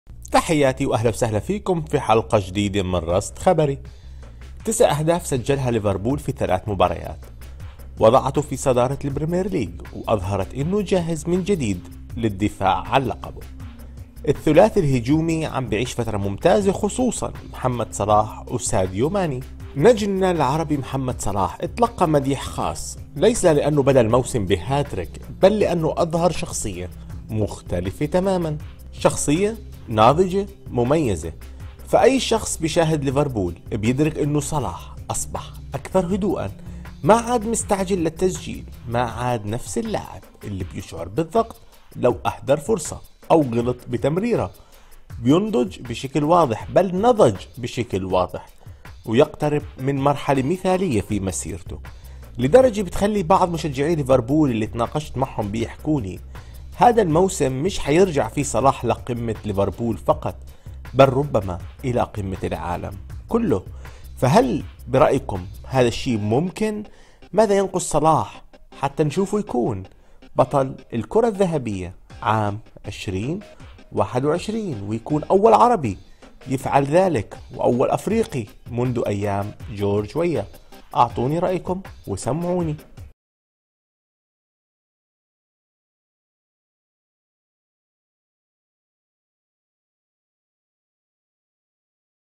محمد صلاح .. نضج مستمر
يواصل محمد صلاح قصة نضج كبير مع  ليفربول .. فأين يصل؟